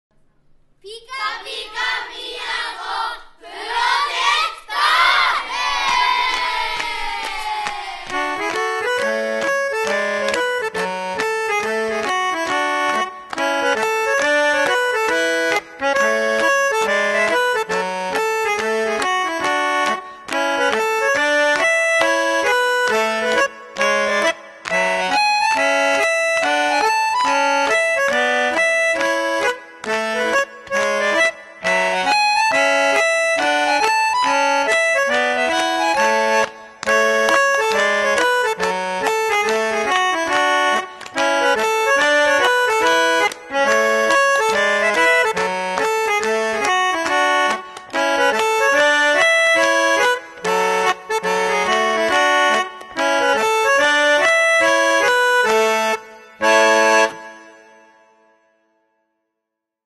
PiKAPiKA 宮古 プロジェクト 鍬ケ崎小学校バージョン
岩手県宮古市の生活復興支援センター、宮古災害復興支援活動チーム M.A.D、大阪電気通信大学、トーチカによってすすめている「PiKAPiKA 宮古 プロジェクト」 の鍬ケ崎小学校バージョン。

震災でバラバラになってしまった、コミュニティの再生による、復興支援になればと思っています。（トーチカナガタ）